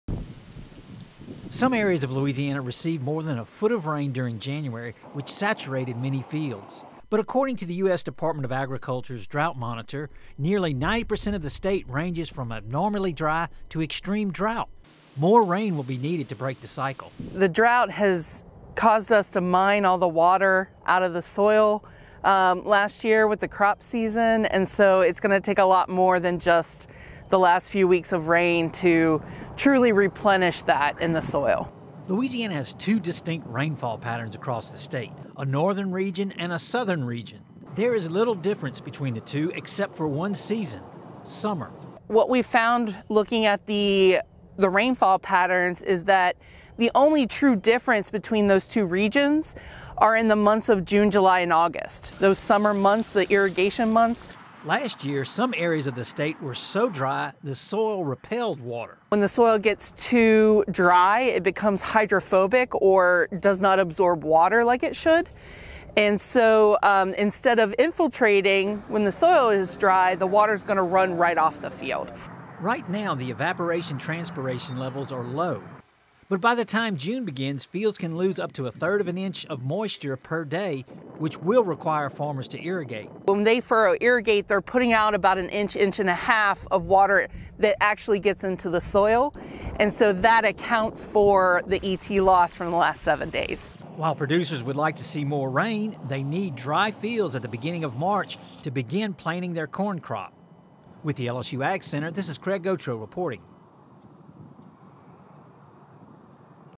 Much needed rain helpful, but more moisture needed to bust drought
In 2023, Louisiana experienced one of its driest and hottest years on record. More seasonal rain patterns have returned the past two months, but the state is still in recovery mode. LSU AgCenter reporter Craig Gautreaux has the story from northwest Louisiana.